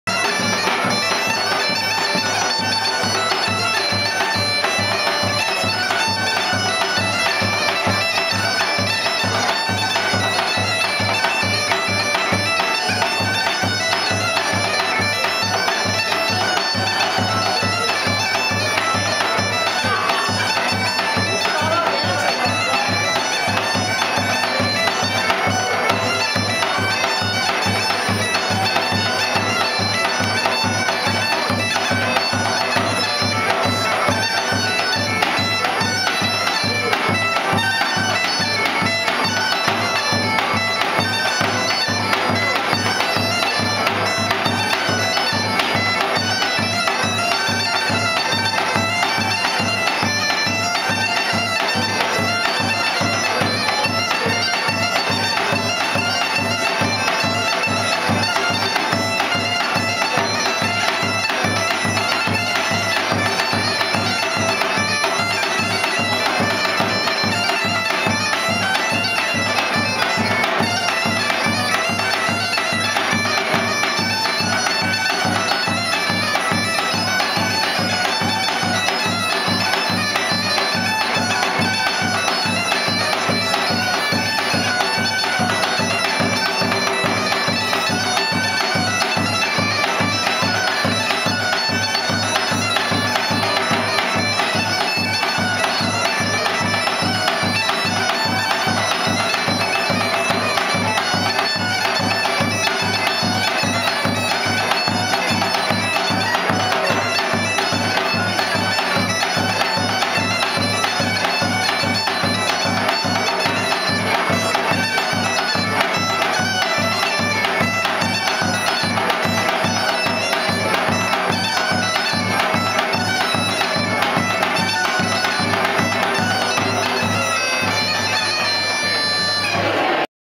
Bulgarian bagpipes in the museum
Mark Levy, University of Oregon professor of Music, plays the gaida (Bulgarian bagpipes) accompanied by Ken Sokolov on drum, at the UO's Museum of Natural and Cultural History, October 9, 2009.   Professor Levy curated the museum's current exhibit "World Harmony :  Musical Instruments from around the Globe".

This music was performed in the museum's new photo gallery; seen behind the pair are images from Brain Lanker's collection "Shall We Dance".